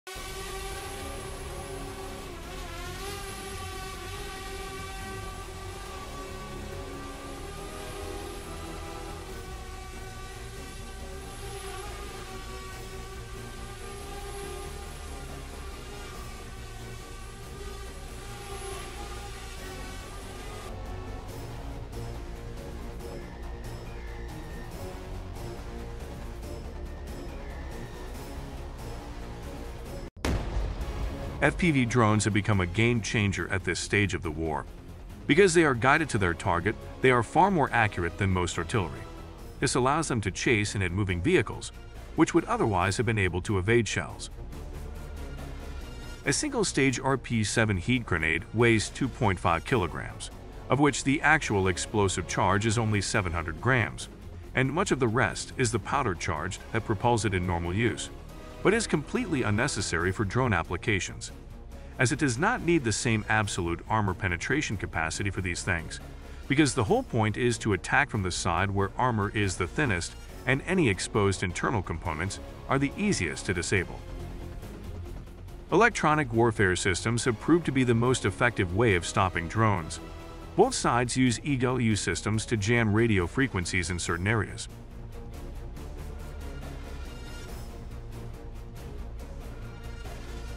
FPV Drone VS Leopard 2AV | Armour Piercing Simulation
This simulation shows the impact of an FPV Drone with PG-7V warhead, guided into the turret side of a German Leopard 2AV tank.

FPV drones have become a game changer at this stage of the war. Because they are guided to their target, they are far more accurate than most artillery. This allows them to chase and hit moving vehicles which would otherwise have been able to evade shells.

A single-stage RPG-7 HEAT grenade weighs 2.5 kg, of which the actual explosive charge is only 700 g, and much of the rest is the powder charge that propels it in normal use, but is completely unnecessary for drone applications. As it does not need the same absolute armor penetration capacity for these things, because the whole point is to attack from the side where armor is the thinnest and any exposed internal components are the easiest to disable.

Electronic warfare (EW) systems have proved to be the most effective way of stopping drones. Both sides use EW systems to jam radio frequencies in certain areas. When a drone's signal is jammed, the pilot loses the ability to control the craft or can no longer see the video signal, depending on which frequency has been disrupted.